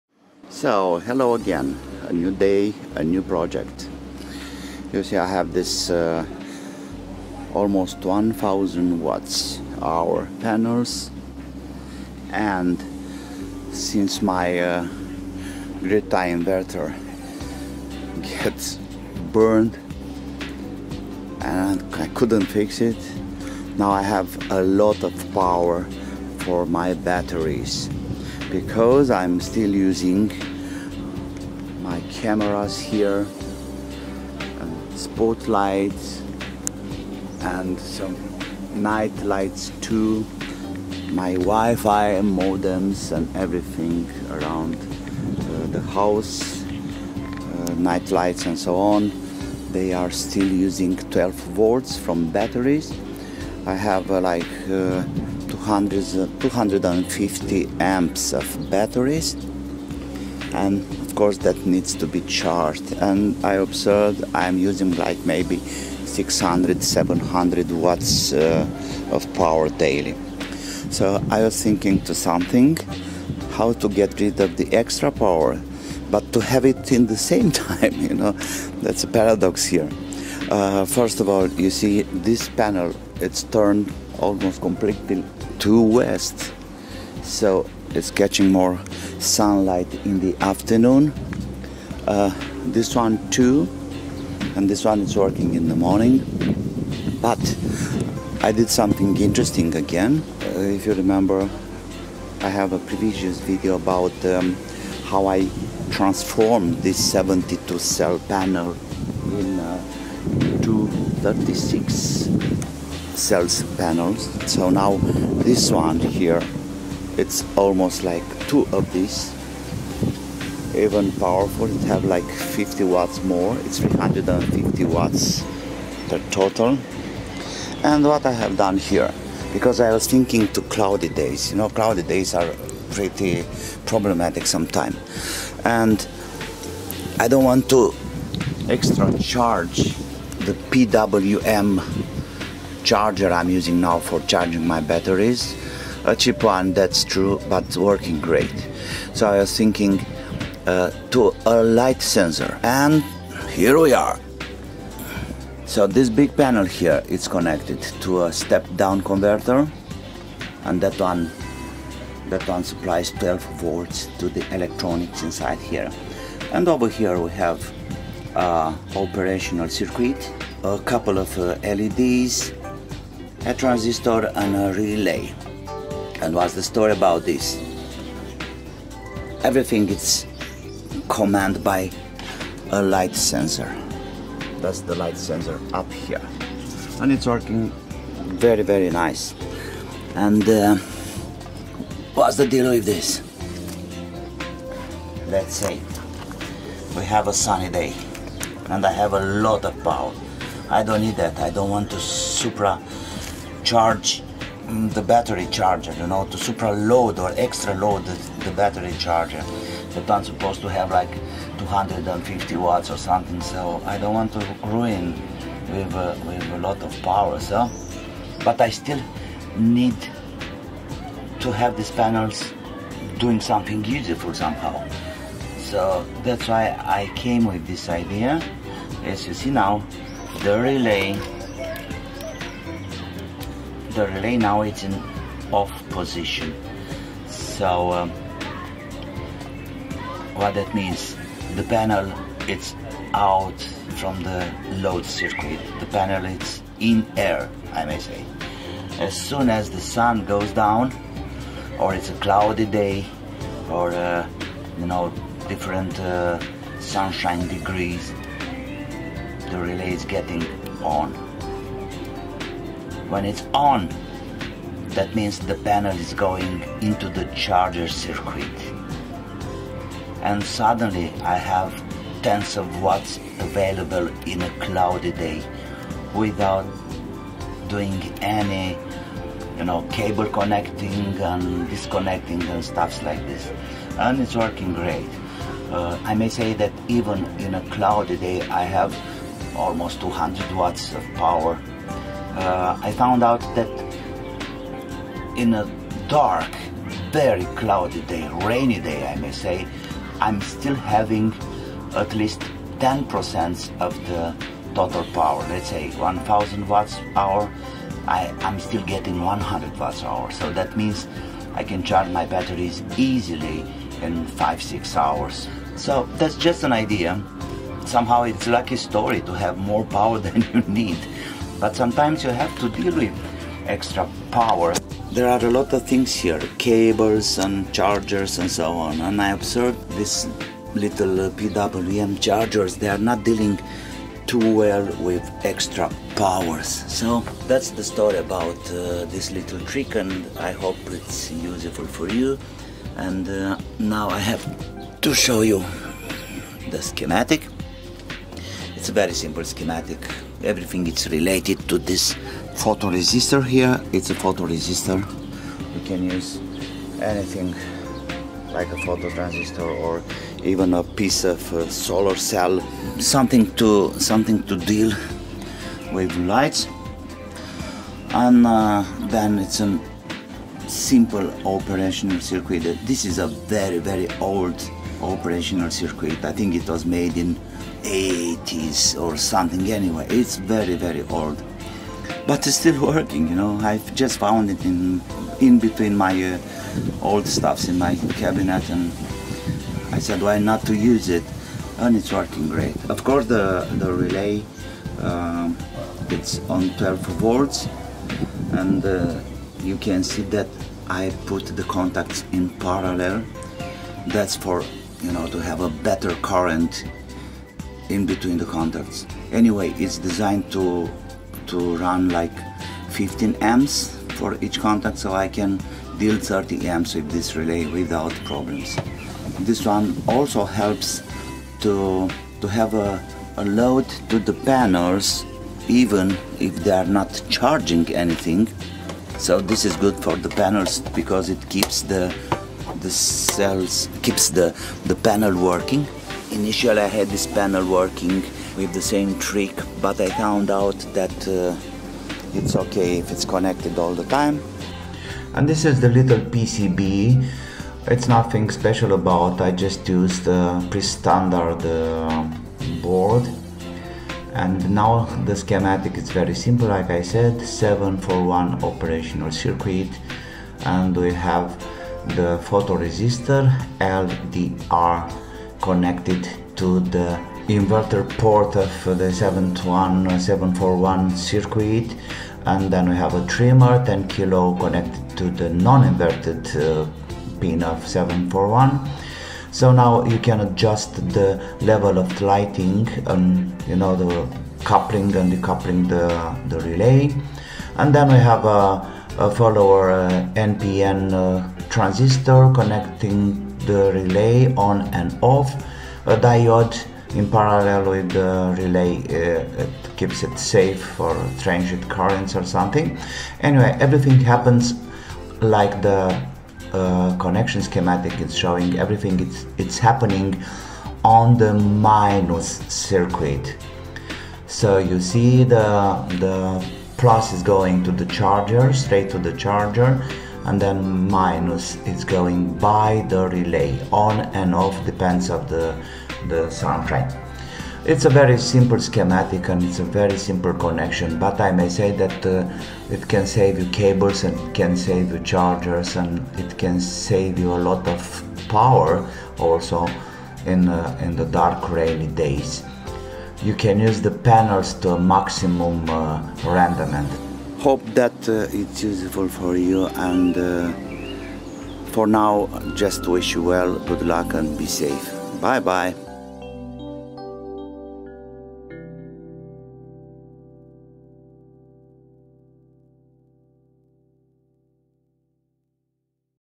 Solar panel mod switch in full sunshine avoid overload
A simple idea how to adapt solar panel  power to PWM charge controller and batteries needs, avoid overload the charger and overcharge batteries, without expensive MPPT devices.